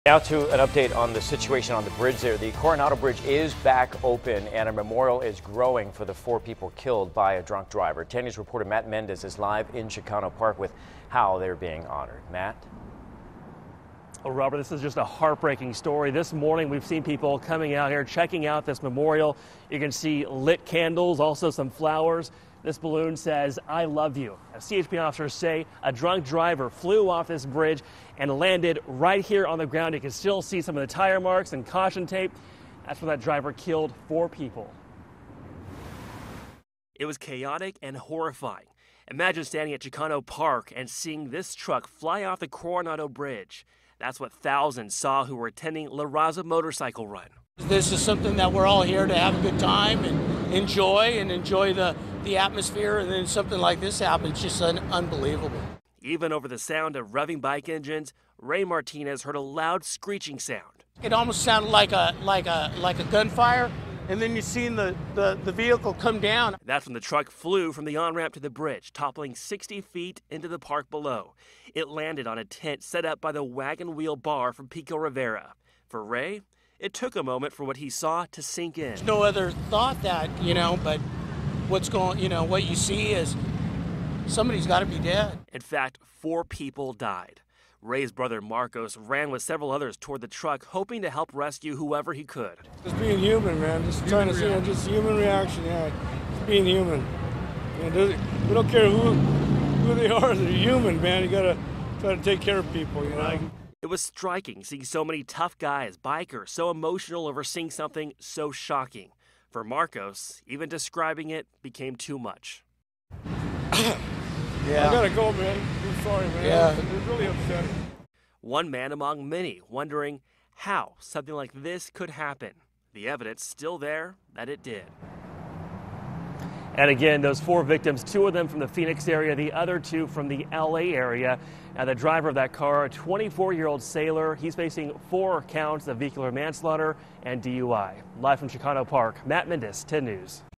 Memorial growing for victims in deadly DUI crash at Chicano Park
A suspected drunk driver flew off the ramp of the Coronado Bridge, killing four people during a crowded event.